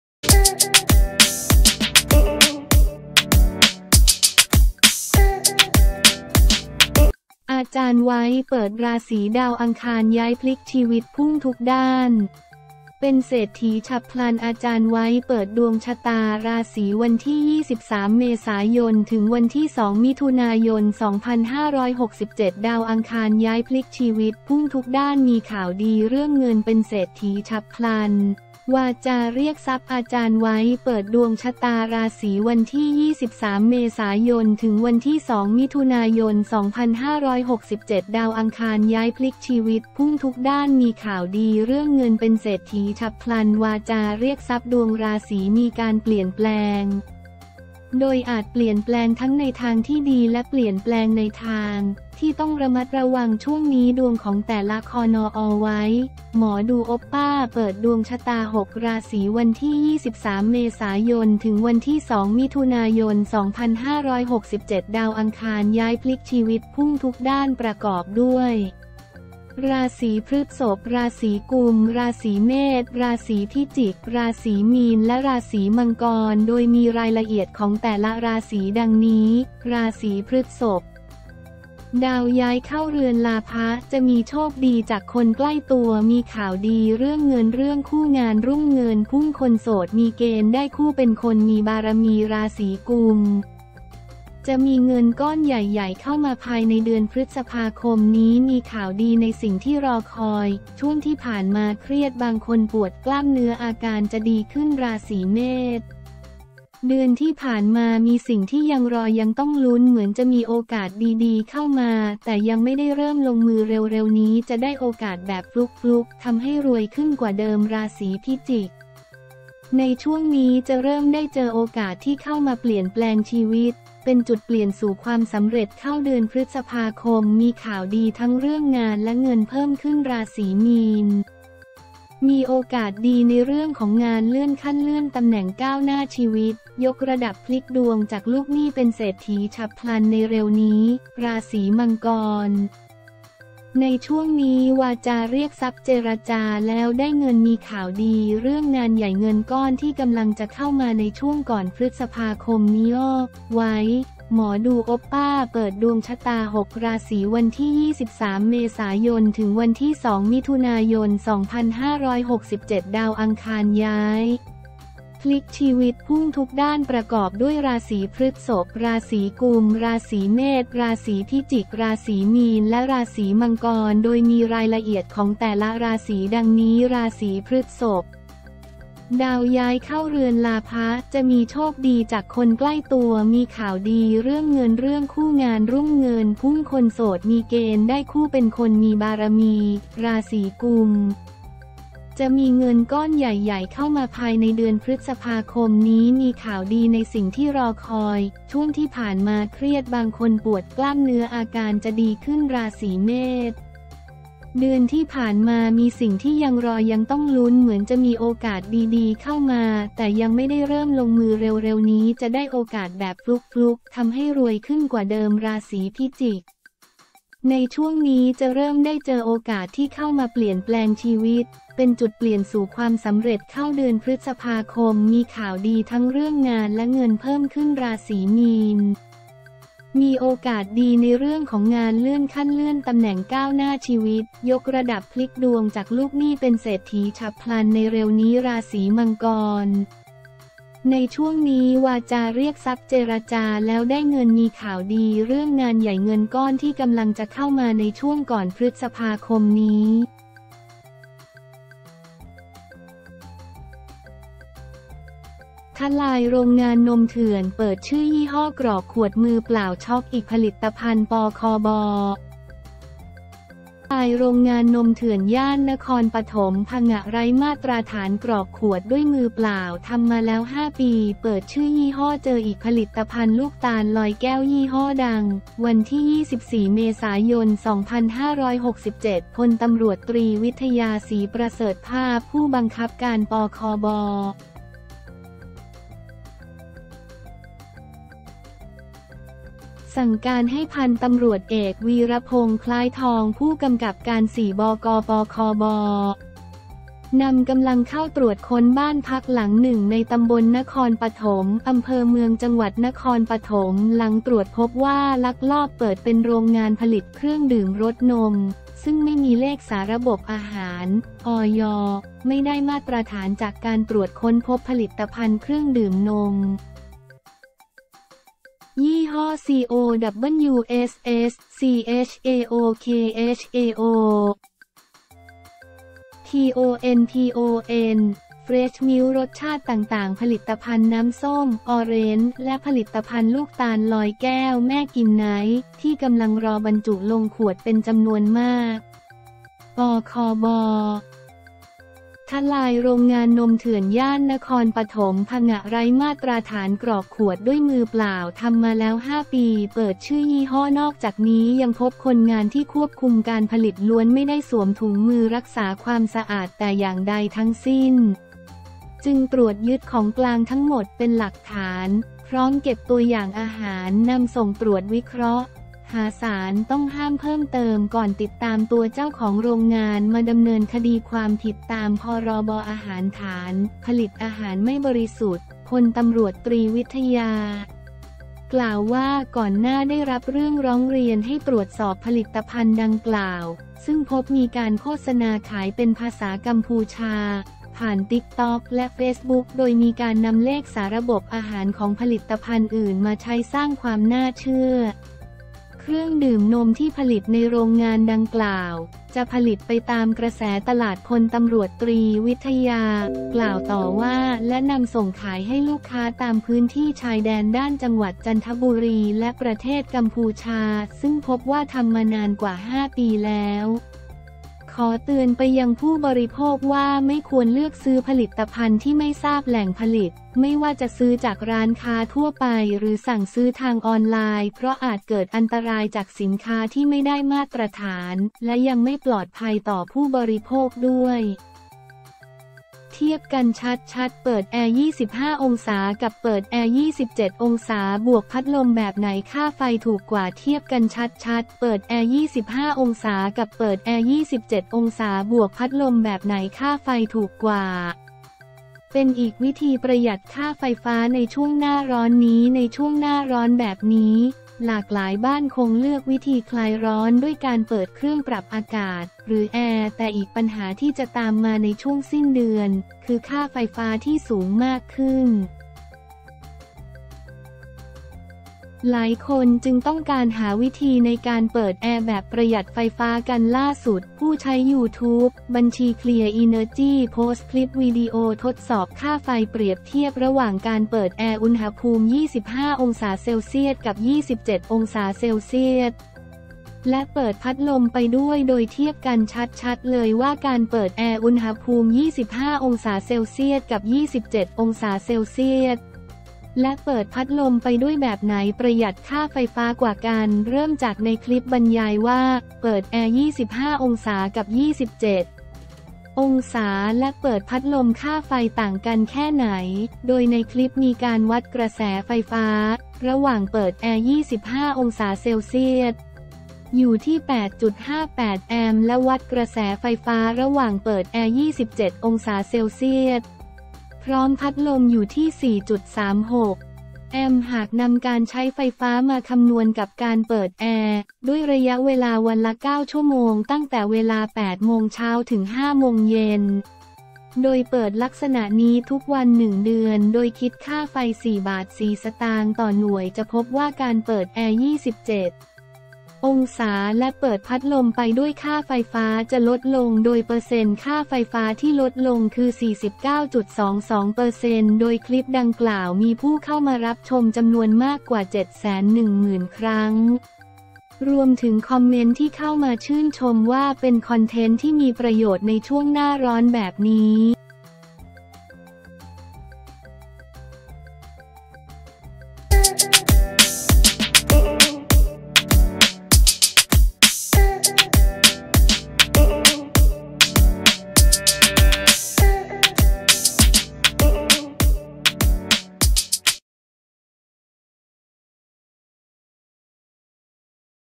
อ.ไวท์ เปิดดวงชะตาราศี 23 เม.ย.-2 มิ.ย.2567 ดาวอังคารย้าย พลิกชีวิตพุ่งทุกด้าน มีข่าวดีเรื่องเงิน เป
อ.ไวท์ เปิดดวงชะตาราศี 23 เม.ย.-2 มิ.ย.2567 ดาวอังคารย้าย พลิกชีวิตพุ่งทุกด้าน มีข่าวดีเรื่องเงิน เป็นเศรษฐีฉับพลัน วาจาเรียกทรัพย์

ดวงราศีมีการเปลี่ยนแปลง โดยอาจเปลี่ยนแปลงทั้งในทางที่ดีและเปลี่ยนแปลงในทาง ที่ต้องระมัดระวังช่วงนี้ดวงของแต่ละคน
อ.ไวท์ หมอดูโอปป้า เปิดดวงชะตา 6 ราศี 23 เม.ย.-2 มิ.ย.2567 ดาวอังคารย้าย พลิกชีวิตพุ่งทุกด้าน ประกอบด้วย ราศีพฤษภ ราศีกุมภ์ ราศีเมษ ราศีพิจิก ราศีมีน และ ราศีมังกร โดยมีรายละเอียดของแต่ละราศีดังนี้

ราศีพฤษภ : ดาวย้ายเข้าเรือนลาภะ จะมีโชคดีจากคนใกล้ตัว มีข่าวดีเรื่องเงิน เรื่องคู่ งานรุ่ง เงินพุ่ง คนโสดมีเกณฑ์ได้คู่เป็นคนมีบารมี
ราศีกุมภ์ : จะมีเงินก้อนใหญ่ ๆ เข้ามา ภายในเดือนพฤษภาคมนี้มีข่าวดีในสิ่งที่รอคอย ช่วงที่ผ่านมาเครียด บางคนปวดกล้ามเนื้อ อาการจะดีขึ้น
ราศีเมษ : เดือนที่ผ่านมา มีสิ่งที่ยังรอยังต้องลุ้น เหมือนจะมีโอกาสดี ๆ เข้ามาแต่ยังไม่ได้เริ่มลงมือ เร็ว ๆ นี้จะได้โอกาสแบบฟลุ๊ก ๆ ทำให้รวยขึ้นกว่าเดิม
ราศีพิจิก : ในช่วงนี้จะเริ่มได้เจอโอกาสที่เข้ามาเปลี่ยนแปลงชีวิต เป็นจุดเปลี่ยนสู่ความสำเร็จ เข้าเดือน พ.ค.มีข่าวดี ทั้งเรื่องงานและเงินเพิ่มขึ้น
ราศีมีน : มีโอกาสดีในเรื่องของงาน เลื่อนขั้น เลื่อนตำแหน่ง ก้าวหน้าชีวิตยกระดับ พลิกดวงจากลูกหนี้เป็นเศรษฐีฉับพลันในเร็วนี้
ราศีมังกร : ในช่วงนี้วาจาเรียกทรัพย์ เจรจาแล้วได้เงิน มีข่าวดีเรื่องงานใหญ่ เงินก้อนที่กำลังจะเข้ามาในช่วงก่อนพฤษภาคมนี้